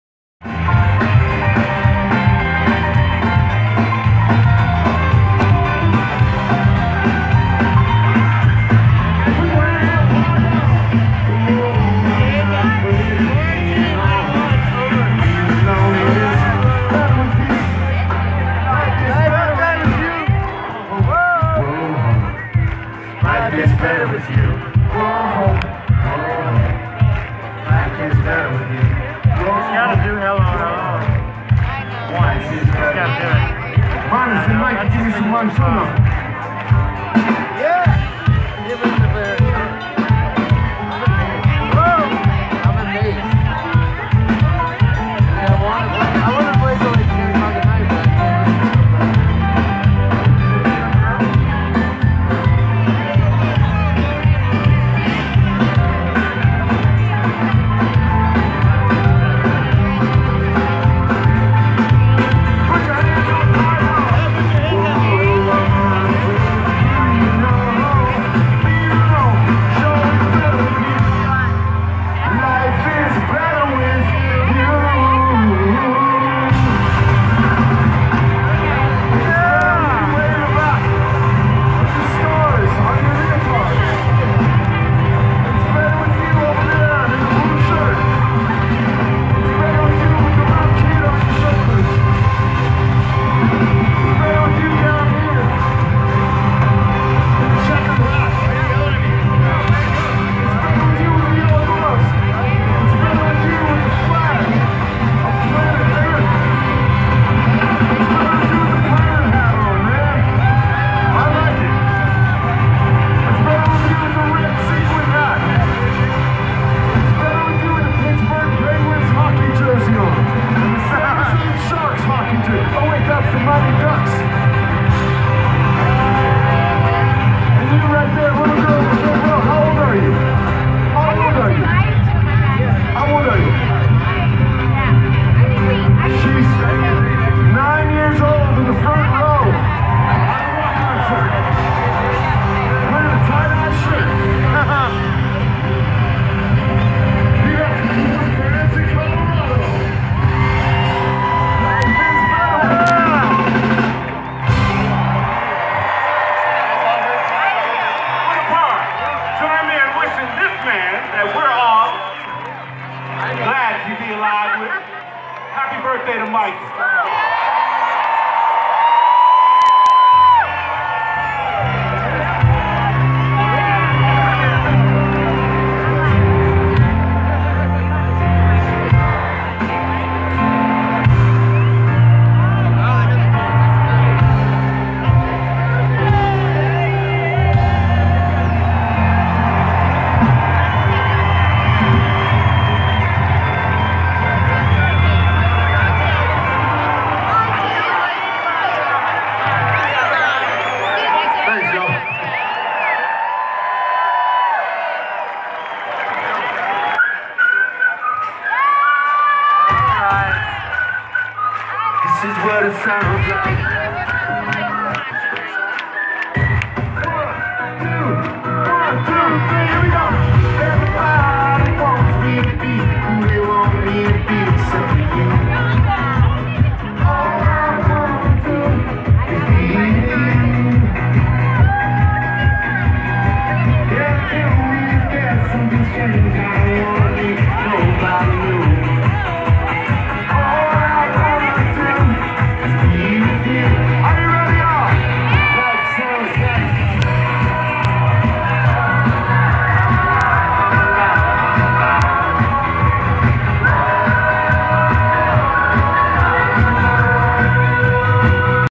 Michael Franti at Springtopia Winter Park, Colorado April 22nd 2017
Michael Franti jamming out in Colorado.